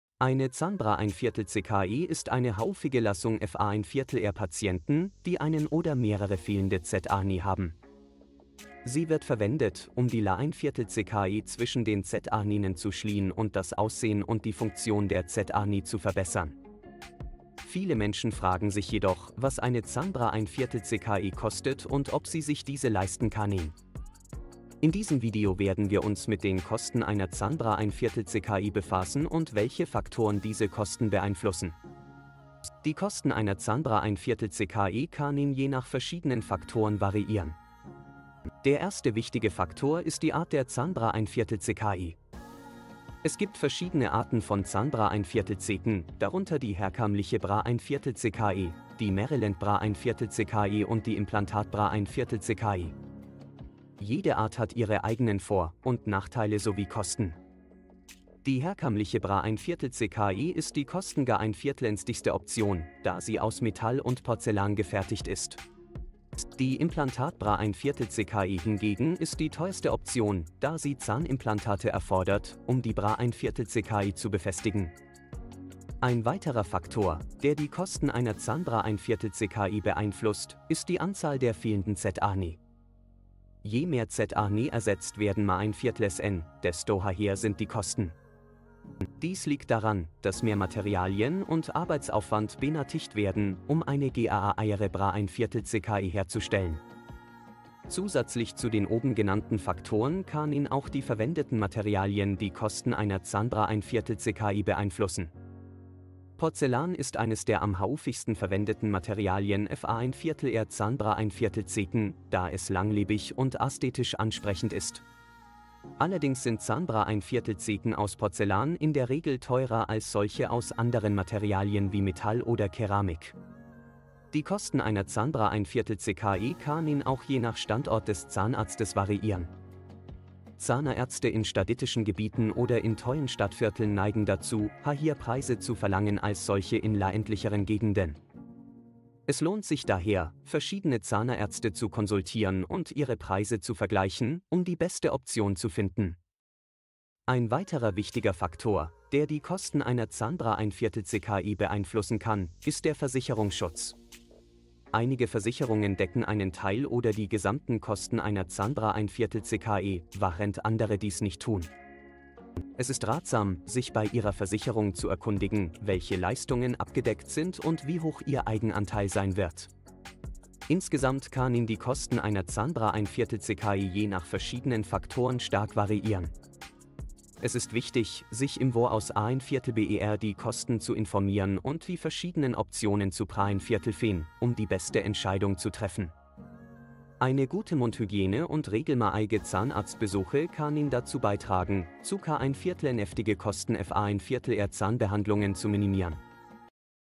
Zahnbrücke: Wie funktioniert sie und wer kann sie bekommen? - Alles, was du über diese Zahnersa
Eine Zahnbrücke ist eine häufige Lösung für Patienten, die einen oder mehrere fehlende Zähne haben. Sie wird verwendet, um die Lücke zwischen den Zähnen zu schließen und das Aussehen und die Funktion der Zähne zu verbessern. Viele Menschen fragen sich jedoch, was eine Zahnbrücke kostet und ob sie sich diese leisten können. In diesem Video werden wir uns mit den Kosten einer Zahnbrücke befassen und welche Faktoren diese Kosten beeinflussen. Zahnbrücke, fehlende Zähne, Lücke, Aussehen, Funktion, verbessern, Patienten, Lösung, häufig, Z